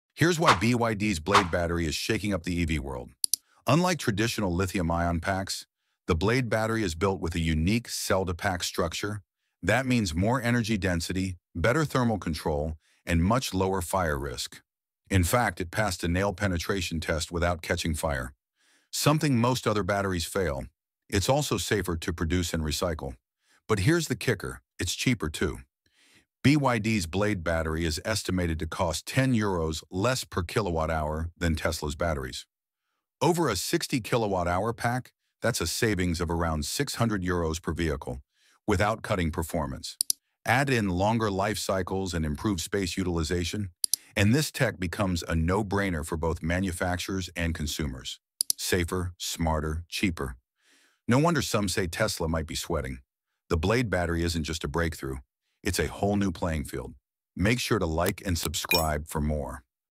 The Blade Battery Explained in 30 Sec
🔋 Blade Battery tech breakdown
🔥 Safety test: nail penetration
💸 Cost savings per vehicle

💬 Is this the future of EV batteries? Let us know!